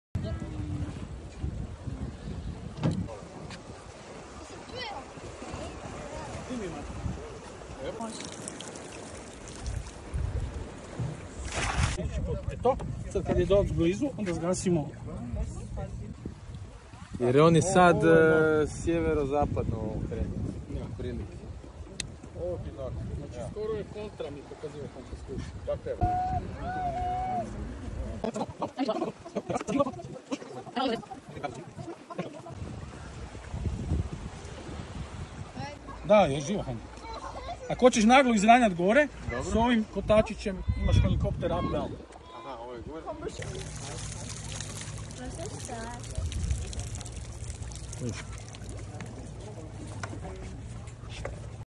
Fifish V-EVO underwater drone, diving with VR goggles, adventures in the crystal Adriatic sea
To learn more about the FIFISH V-EVO drone, please visit  ➤

Thanks to IG@andrea09pavic & @diveitcroatia for the cool shots and share 📷